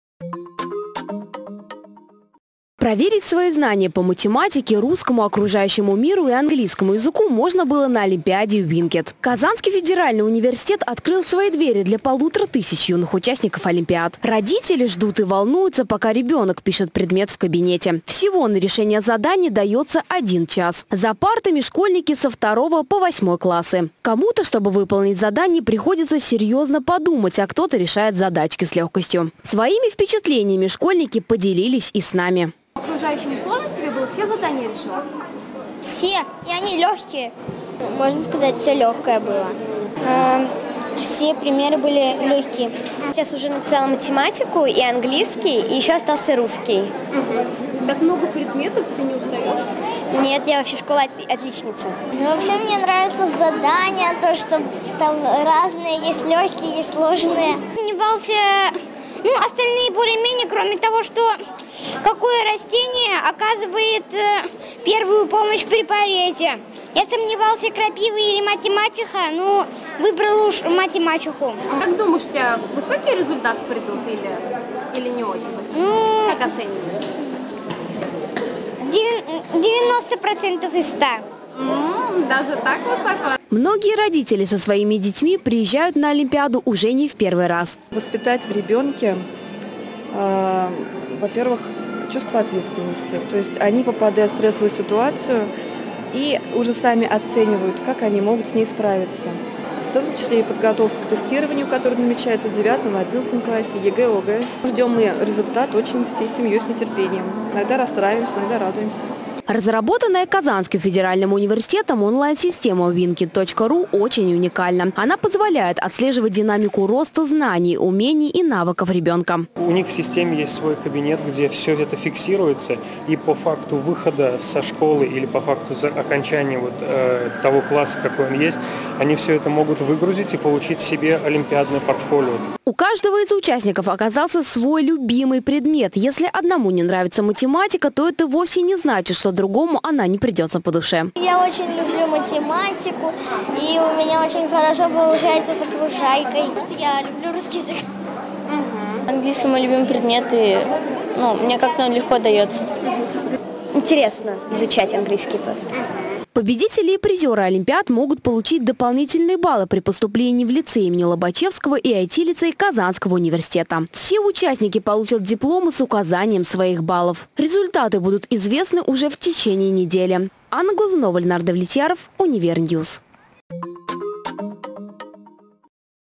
Школьники за партами Казанского университета
Приучать к труду и решению задач в стрессовой ситуации необходимо ребенка с детства! Участие в олимпиадах с раннего возраста только положительно скажется на успеваемости школьника, - считают родители и организаторы. С юными участниками пообщалась и наш корреспондент Анна Глазунова и узнала их мнение на этот счет.